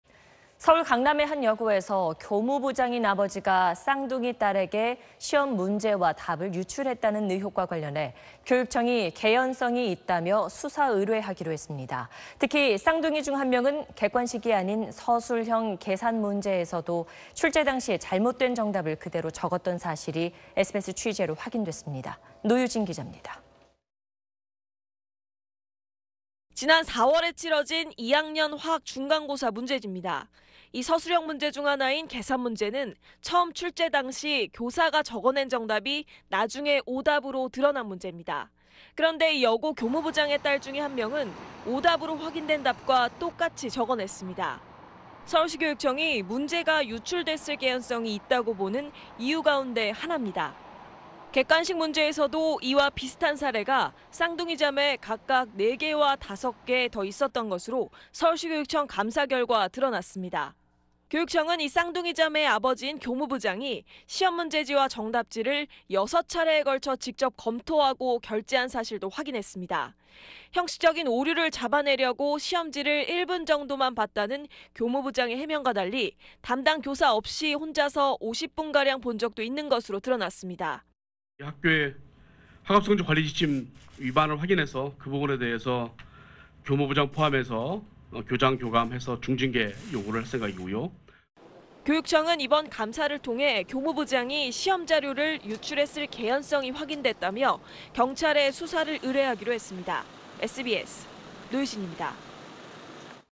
"서술형 오답마저 일치"…'시험지 유출 의혹' 수사 의뢰 / SBS
강남의 한 여고에서 교무부장인 아버지가 쌍둥이 딸에게 시험문제와 답을 유출했다는 의혹과 관련해 교육청이 개연성이 있다며 수사 의뢰하기로 했습니다. 특히 쌍둥이 중 한 명은 객관식이 아닌 서술형 계산 문제에서도 출제 당시의 잘못된 정답을 그대로 적었던 사실이 SBS 취재로 확인됐습니다.
노유진 기자입니다.